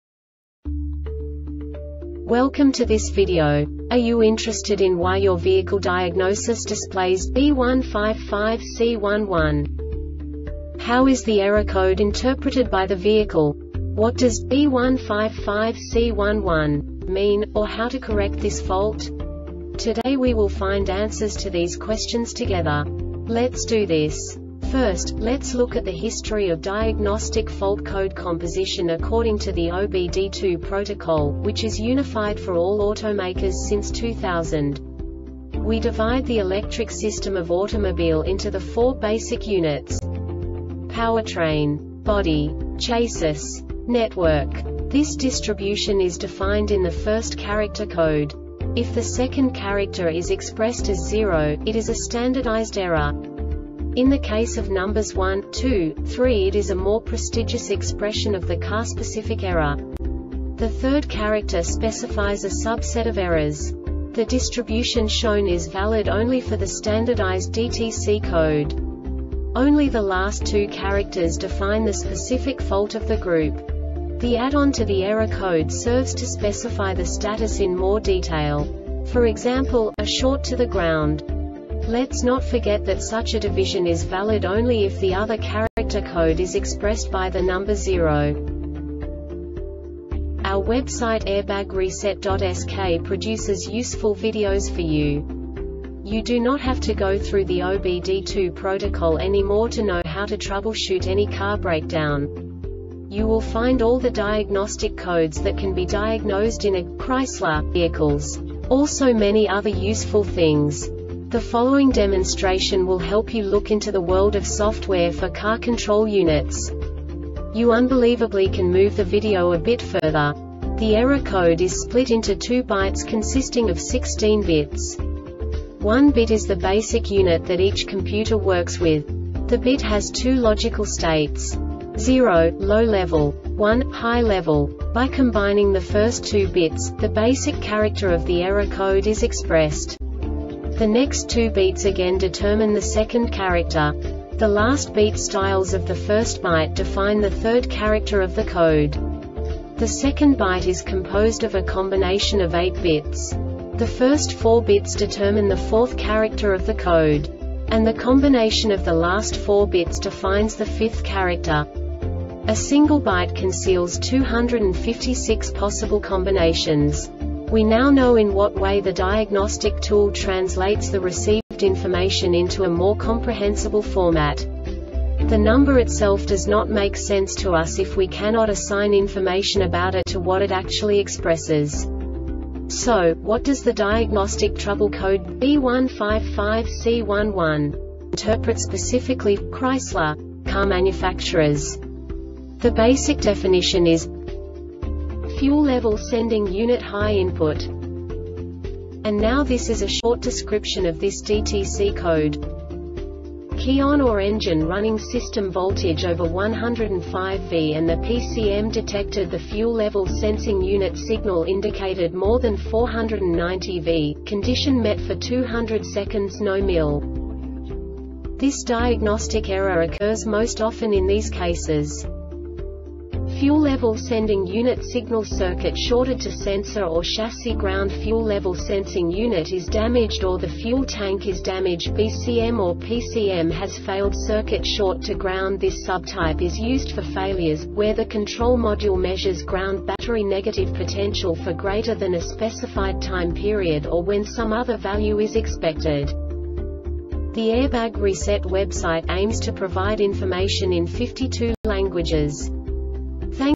DTC Chrysler B155C-11 Short Explanation
Contents:
0:21 Basic DTC analysis according to OBD2 protocol standard.
1:48 Insight into programming
2:58 A diagnostically understandable description of the Diagnostic Trouble Code
3:05 Basic error definition

Make:
Chrysler
Code:
B155C 11
Definition:
Fuel Level Sending Unit High Input
Description:
Key on or engine running;system voltage over 10.5v and the PCM detected the fuel level sensing unit signal indicated more than 4.90v,condition met for 200 seconds.No MIL.
Cause:
Fuel level sending unit signal circuit shorted to Sensor or chassis groundFuel level sensing unit is damaged or the fuel tank is damagedBCM or PCM has failed
Failure Type: